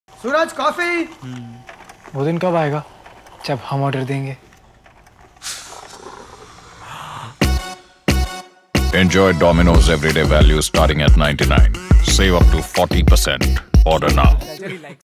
Boss level fun with Domino's EverydayValueOffer!
Your wish is our command! Place your order with us and get it delivered within 30 minutes! Order our delicious range of pizzas from EverydayValueOffer starting at INR 99/- each and save up to 40% on your orders.